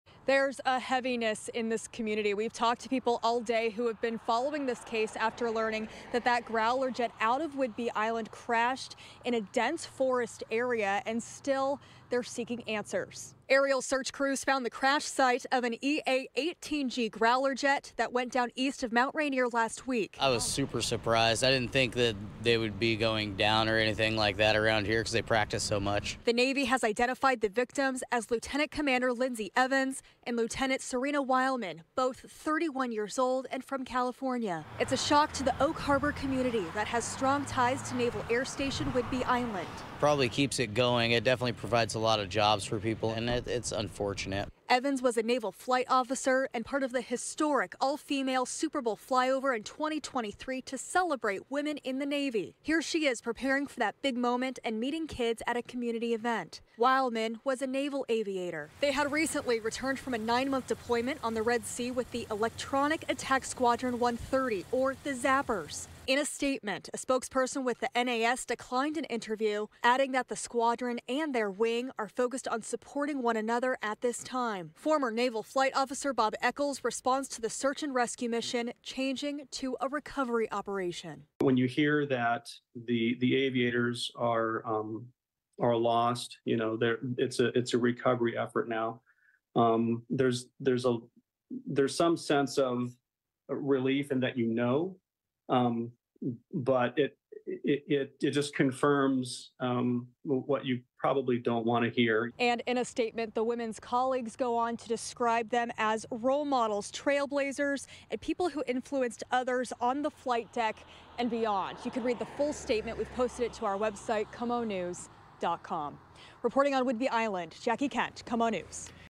US Navy identifies 2 aviators killed in EA-18G Growler jet crash near Mount Rainier
The U.S. Navy has identified the two aviators who died when their EA-18G Growler aircraft crashed near Mount Rainier last week.

Lt. Cmdr. Lyndsay P. Evans and Lt. Serena N. Wileman were declared dead Sunday by the Navy and search and rescue efforts were shifted to recovery operations.

Evans was a naval flight officer and Wileman was a naval aviator. Both were 31 years old and from California.

Investigators are still working to learn what led up to the Oct. 15 crash during a "routine training flight."

Jackie Kent